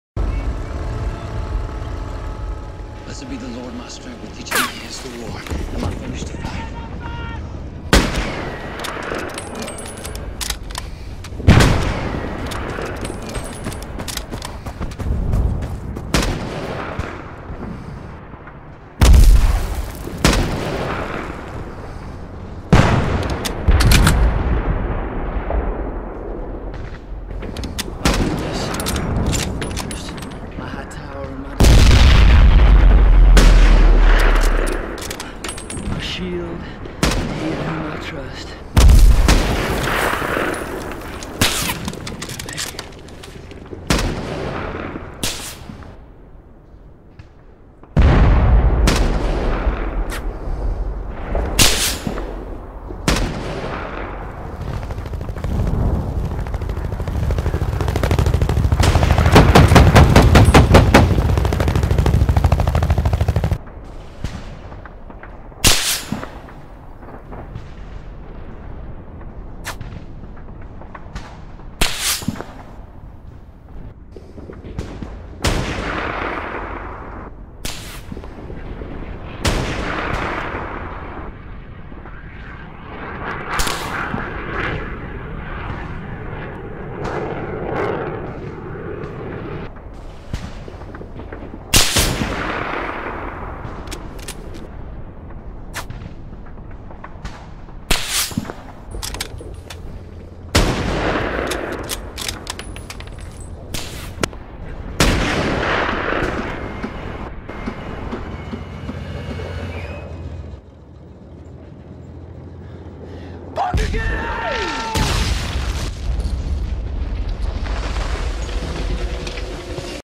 Saving Private Talron
Having fun sniping clueless NC on top of the scarred mesa tower with only my trusty Rams .50M

Tribute to Berry Pepper as he portrays Private Daniel Jackson from saving private Ryan